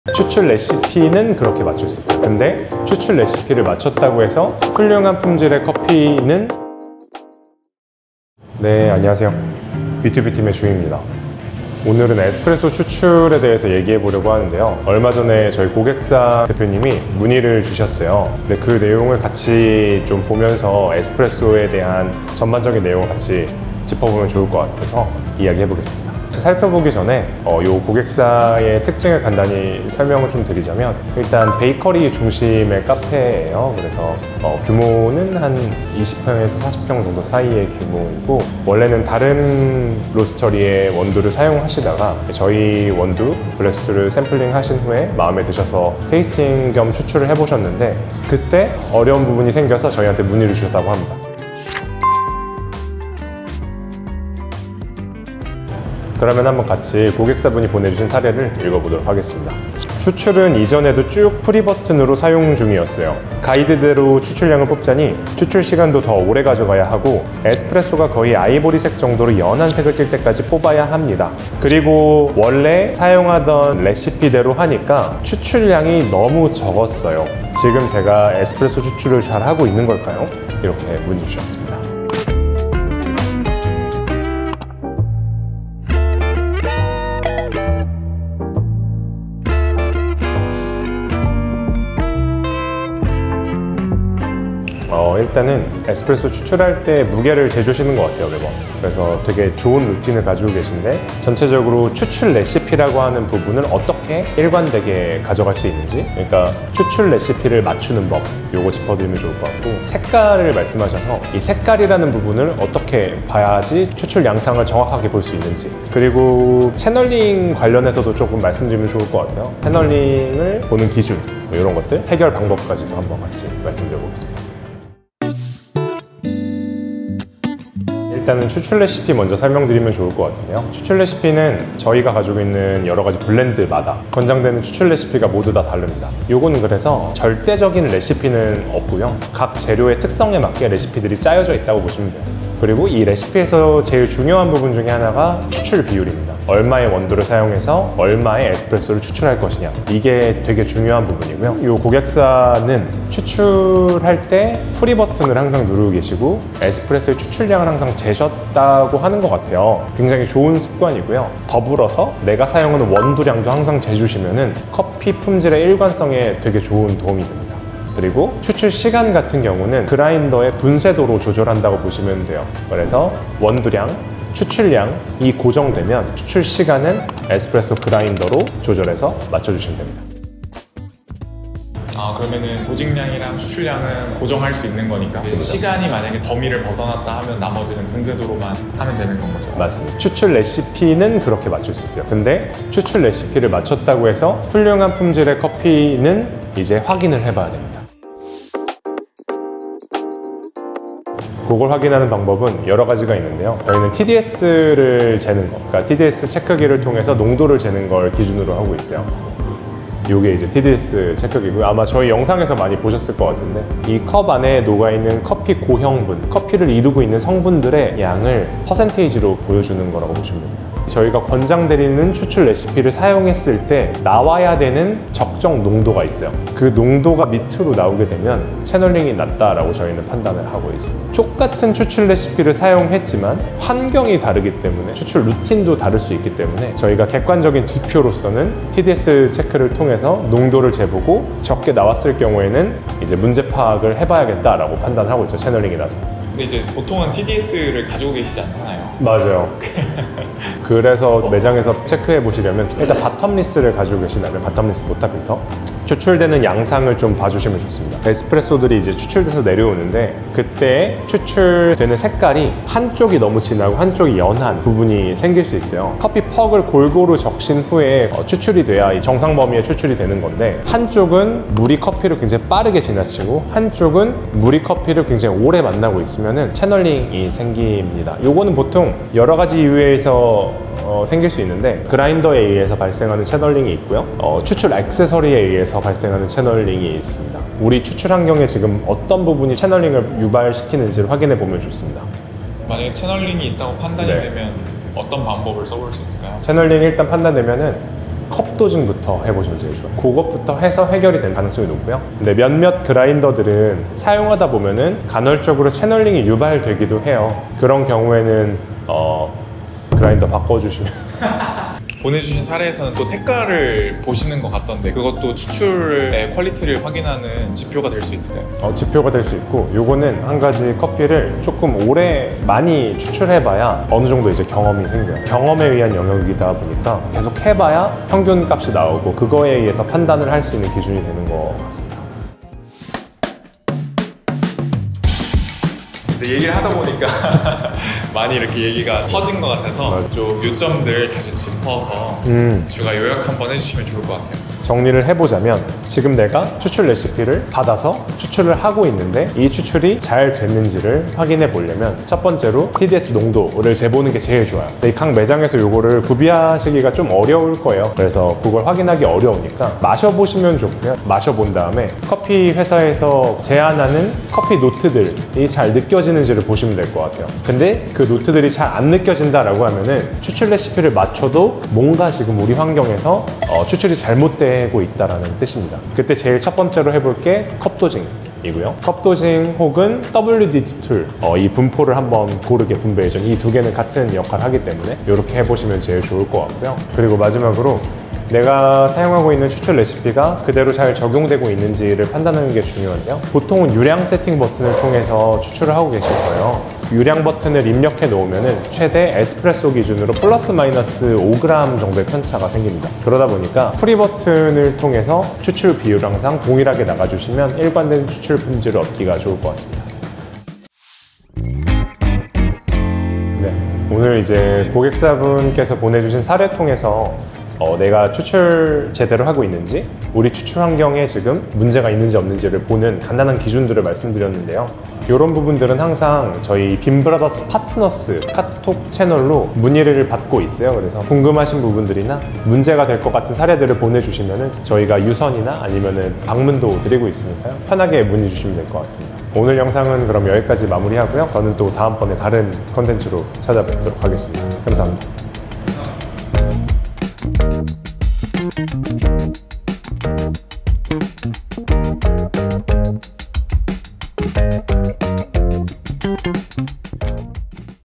제대로 된 에스프레소 추출, 이것까지 보셔야 해요ㅣ고객사 문의 사례로 보는 추출 노하우
얼마 전 고객사에서 에스프레소 추출에 어려움을 느껴 문의를 주셨는데요.
주세훈(Ju) 팀장의 노하우 전수로 이제는 무리없이 맛있는 커피를 추출하고 계신다고 합니다!
여러분과 함께 나누면 좋을 것 같아 영상으로 찍어보았는데요. 함께 볼까요~?
빈브라더스 파트너스 소개 페이지
00:00 인트로
00:08 안녕하세요
00:56 고객사 문의 내용 살펴보기
01:30 chapter.1 어떻게 해결할 수 있나요?
02:05 chapter.2 추출 레시피 바로잡기
03:18 chapter.3 커피 퀄리티 체크하기
05:32 chapter.4 그래서 중요한 것은
06:50 Closing. 무엇이든 물어보세요!